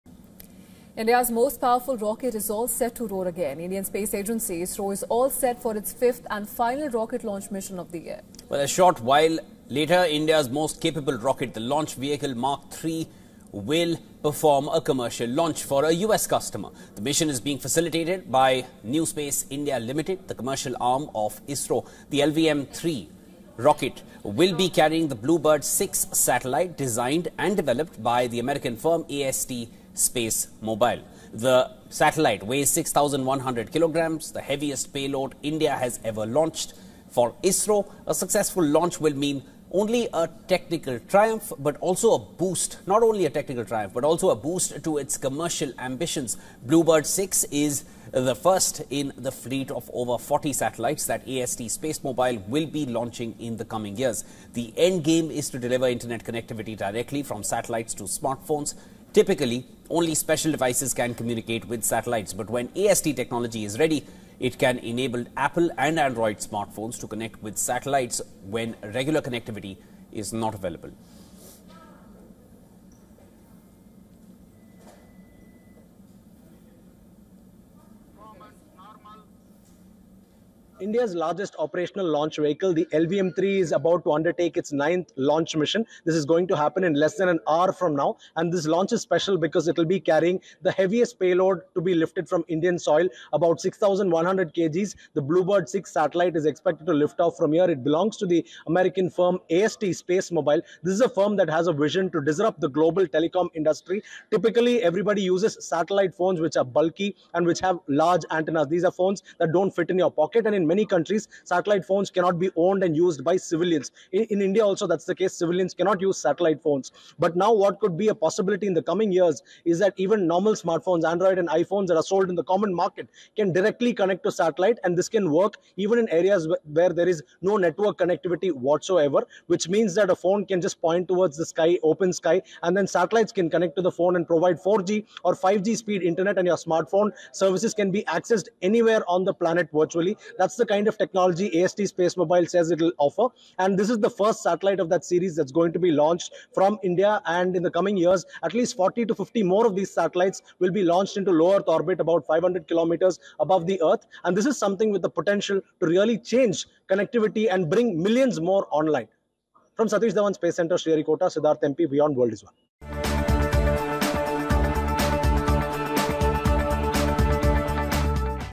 BlueBird-6 Satellite: India’s ISRO to Launch AST SpaceMobile’s 6,100 kg BlueBird-6 | WION
India’s ISRO is set for its largest-ever rocket launch, deploying the 6,100 kg BlueBird-6 satellite for US-based AST SpaceMobile. The mission, scheduled to lift off soon, aims to provide space-based cellular broadband directly to smartphones worldwide. This launch marks a major milestone in commercial space collaboration and global connectivity efforts led by India’s space program.